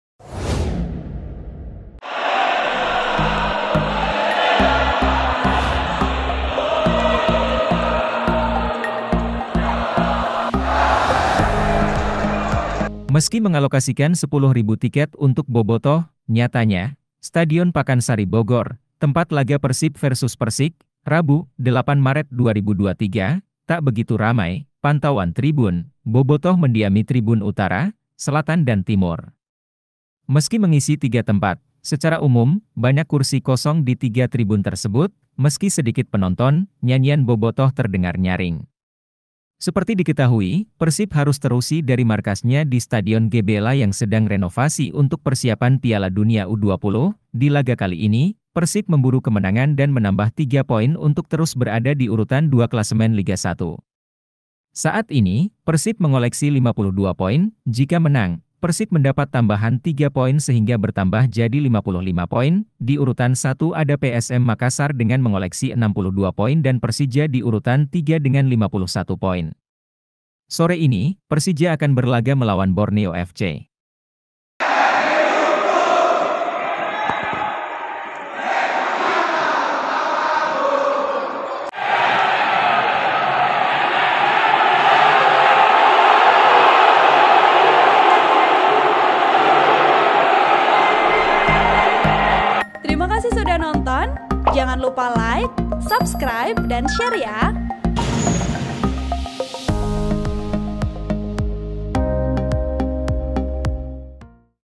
KASIHAN PERSIB TERUSIR DARI GBLA, STADION PAKANSARI SEPI SAAT LAWAN PERSIK KEDIRI
SUASANA STD PAKANSARI LAGA PERSIB

TRIBUNJABARVIDEO- Meski mengalokasikan 10 ribu tiket untuk bobotoh, nyatanya, Stadion Pakansari Bogor, tempat laga Persib vs Persik, Rabu (8/3/2023) tak begitu ramai.

Pantauan Tribun, bobotoh mendiami tribun utara, selatan dan timur. Meski mengisi tiga tempat, secara umum, banyak kursi kosong di tiga tribun tersebut.

Meski sedikit penonton, nyanyian bobotoh terdengar nyaring. Seperti diketahui, Persib harus terusi dari markasnya di Stadion GBLA yang sedang renovasi untuk persiapan Piala Dunia U 20.

Di laga kali ini, Persib memburu kemenangan dan menambah 3 poin untuk terus berada di urutan 2 klasemen Liga 1. Saat ini, Persib mengoleksi 52 poin.

Jika menang, Persib mendapat tambahan 3 poin sehingga bertambah jadi 55 poin.

Di urutan 1 ada PSM Makassar dengan mengoleksi 62 poin dan Persija di urutan 3 dengan 51 poin. Sore ini, Persija akan berlaga melawan Borneo FC.*

Editor Mega Nugraha | Video Editor: Edwin Tk